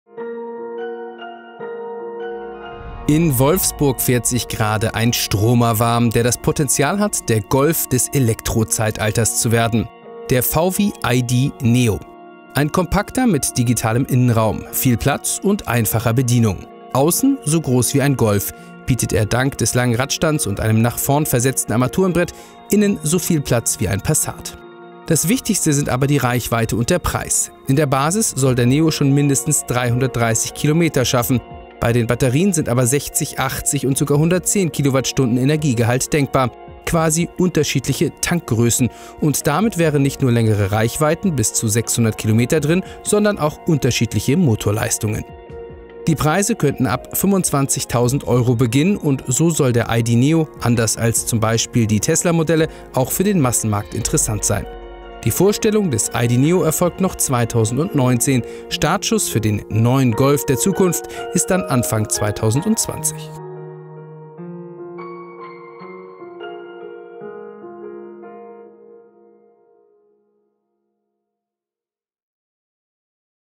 Zukunftsaussicht: VW I.D. Neo (2020) Details / Erklärung
VWs ID-Studie wird 2019 zum Serienauto. Das erste reine Elektro-Auto aus Wolfsburg soll eine Reichweite von mindestens 330 Kilometern schaffen.